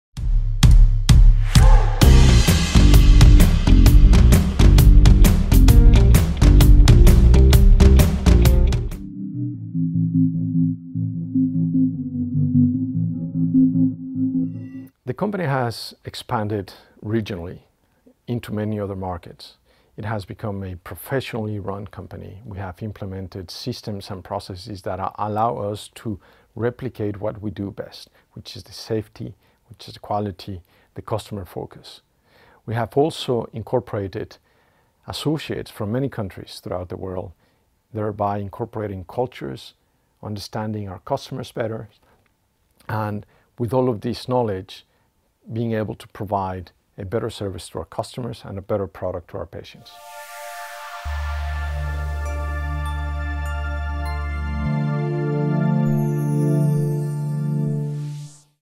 🎙️ Since you've worked at GCA, how has the company evolved?
We asked Bernardo Garduño, General Manager of North Latin American & Asia-Pacific, how GC Aesthetics has evolved during his tenure.